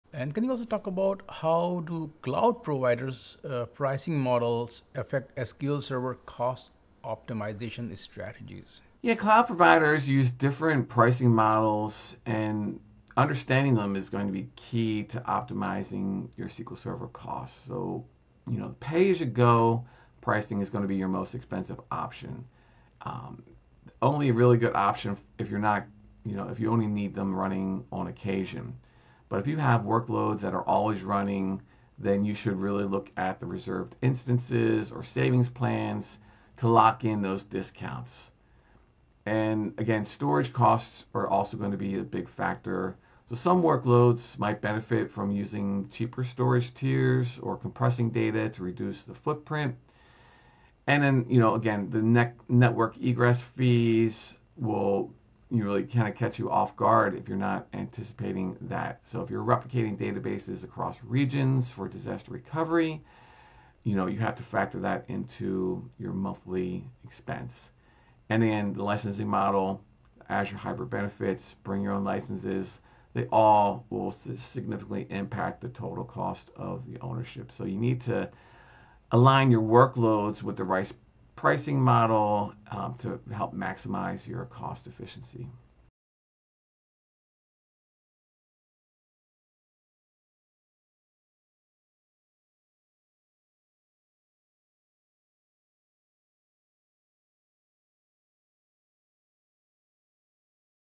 How to Avoid Hidden Cloud Costs in SQL Server Deployments
Cloud pricing models can make or break your SQL Server cost optimization strategy—and understanding the nuances is critical for DevOps, DBAs, and IT decision-makers. In this episode of TFiR, Dave Bermingham, Director of Customer Success at SIOS Technology, breaks down how different cloud pricing models—from Pay-As-You-Go to Reserved Instances—can drastically influence your SQL Server total cost of ownership (TCO).

Bermingham shares actionable insights on when to use cheaper storage tiers, how to handle network egress fees, and why licensing models like Azure Hybrid Benefits matter more than you think. With SQL Server workloads continuing to migrate to the cloud, this conversation is essential for teams looking to optimize performance without breaking the budget.

📺 Watch now to learn how aligning your workloads with the right cloud pricing model can save both money and operational headaches.